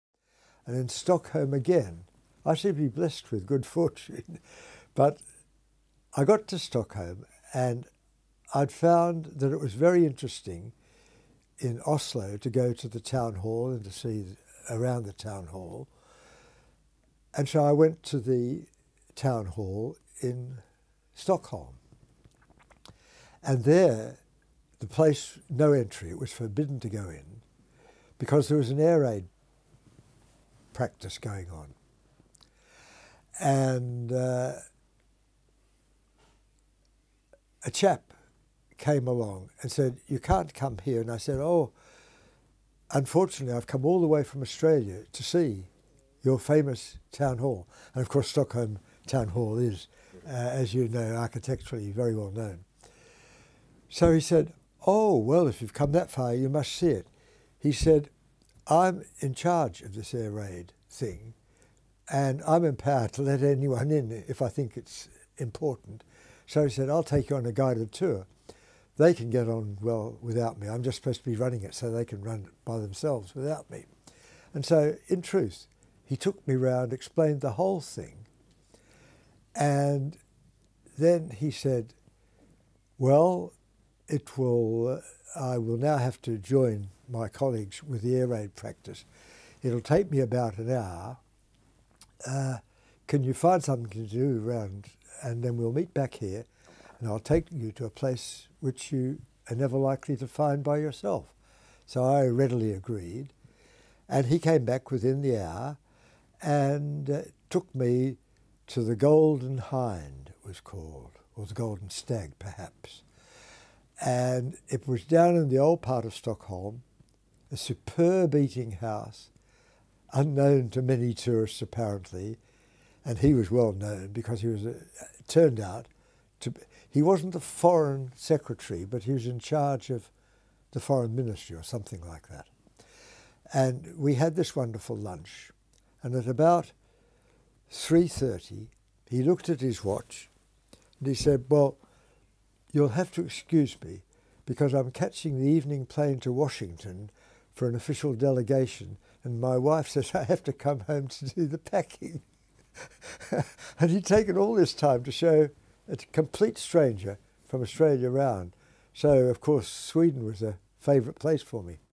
Norman Greenwood - My VIP Stockholm tourist guide (62/252)
Born in Australia, the late Norman Greenwood (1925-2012) was an inorganic chemist whose research led to major advances in the chemistry of boron hydrides and pioneered the application of Mössbauer spectroscopy in the field of chemistry. [Listener: Brian Johnson; date recorded: 2011]

TRANSCRIPT: And in Stockholm again – I seemed to be blessed with good fortune – but I got to Stockholm and I'd found that it was very interesting in Oslo to go to the Town Hall and to see around the Town Hall, and so I went to the Town Hall in Stockholm. And there the place... No entry, it was forbidden to go in because there was an air raid practice going on, and a chap came along and said, 'You can't come here'. And I said, 'Oh, unfortunately I've come all the way from Australia to see your famous Town Hall', and, of course, Stockholm Town Hall is, as you know, architecturally very well known. So he said, 'Oh, well, if you've come that far, you must see it'. He said, 'I'm in charge of this air raid thing, and I'm empowered to let anyone in if I think it's important'. 'So', he said, 'I'll take you on a guided tour. They can get on well without me, I'm just supposed to be running it, so they can run by themselves, without me'. And so, in truth, he took me round, explained the whole thing, and then he said, 'Well, it will... I will now have to join my colleagues with the air raid practice. It will take me about an hour. Can you find something to do around... and then we'll meet back here and I'll take you to a place which you are never likely to find by yourself'.

So I readily agreed, and he came back within the hour and took me to 'The Golden Hind', it was called, or 'The Golden Stag', perhaps, and it was down in the old part of Stockholm, a superb eating-house unknown to many tourists, apparently. And he was well known because he was, turned out to be... he wasn't the Foreign Secretary, but he was in charge of the Foreign Ministry or something like that. And we had this wonderful lunch, and at about 3.30 he looked at his watch and he said, 'Well, you'll have to excuse me, because I'm catching the evening plane to Washington for an official delegation, and my wife says I have to come home to do the packing'. And he'd taken all this time to show a complete stranger from Australia around! So of course Sweden was a favourite place for me.